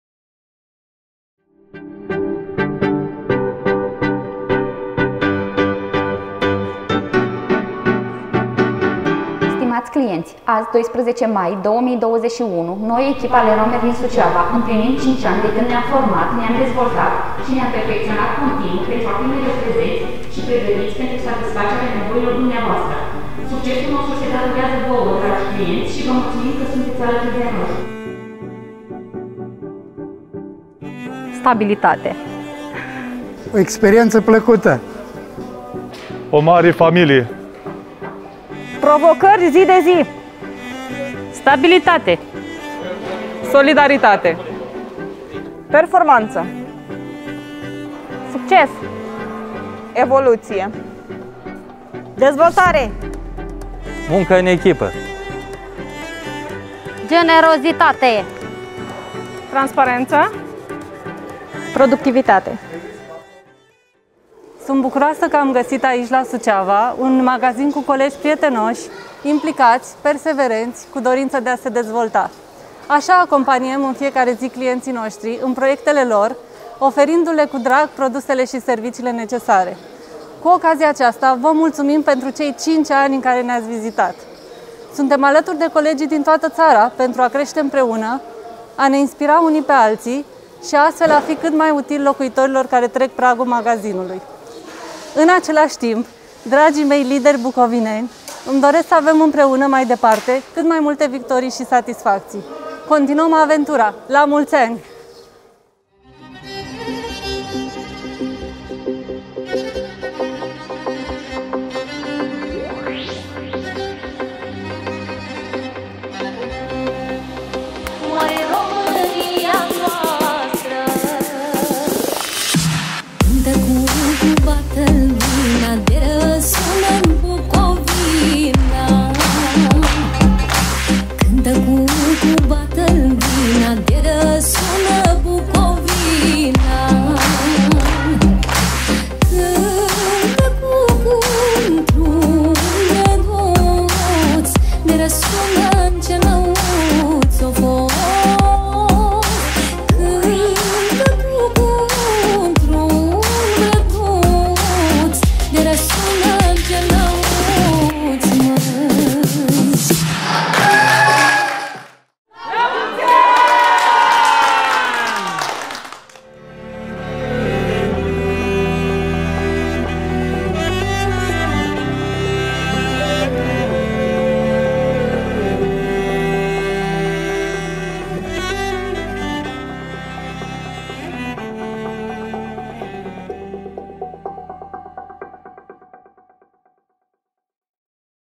Aniversare 5 ani - Magazinul LEROY MERLIN Suceava
Colegialitate, spirit de echipa, incredere si devotament - asa se poate defini in doar cateva cuvinte familia LEROY MERLIN Suceava.

Impreuna suntem creatori de momente, proiecte, strategii si povesti, iar dorinta noastra este sa contribuim permanent la imbunatatirea vietii pentru toti locuitorii planetei.

Pe 12 mai 2021 colegii din Suceava sarbatoresc cea de-a 5-a aniversare. La multi ani!